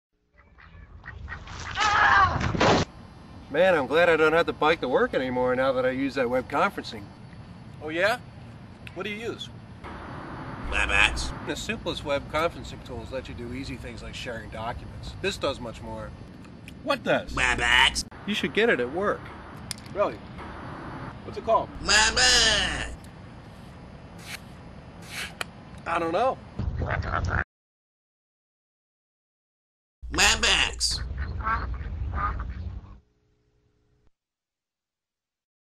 Aflac Park Bench 3M
So we put together some knock-offs of popular commercials. My favorite is the "WebEx" knock-off of the ever-popular AFLAC series.